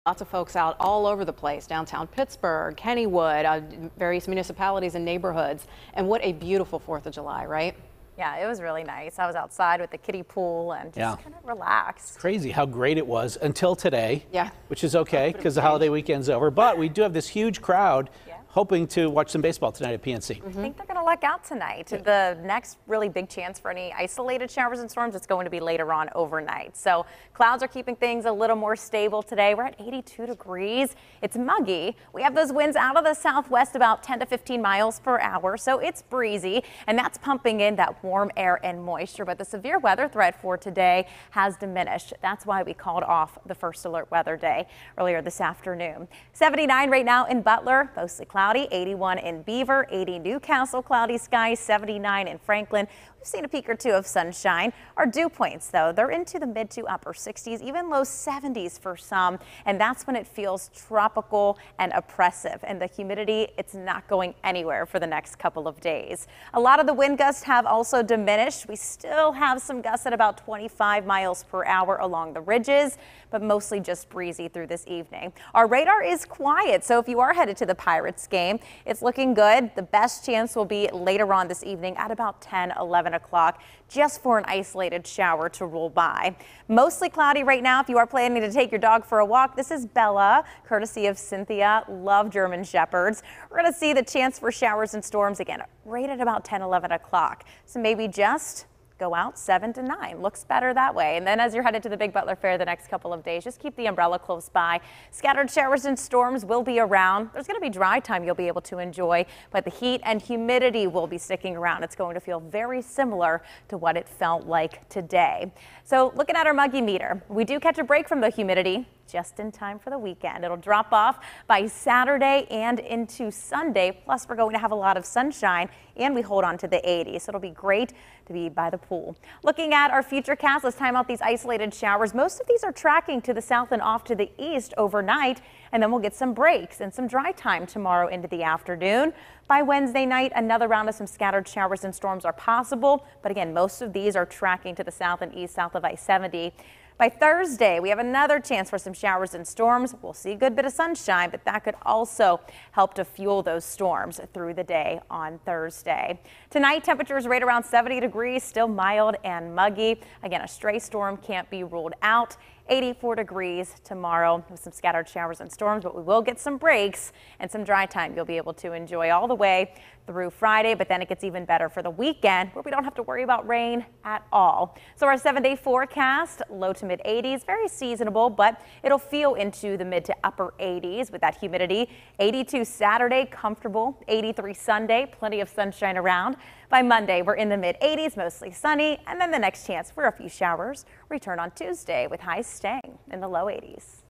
KDKA-TV Evening Forecast (7/5)
Stay on top of local weather with meteorologist Mary Ours' 7-day forecast!